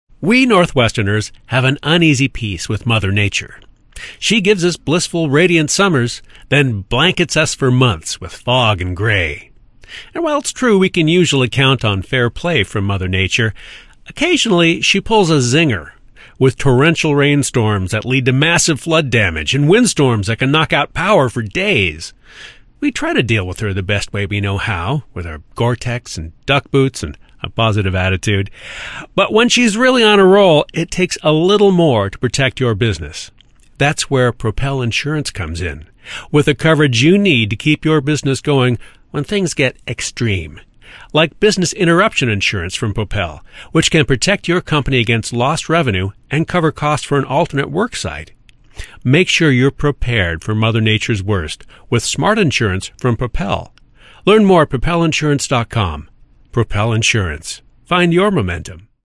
Radio Ad: Mother Nature's Worst
We Northwesterners have an uneasy peace with Mother Nature. She gives us blissful, radiant summers, then blankets us for months with fog and gray.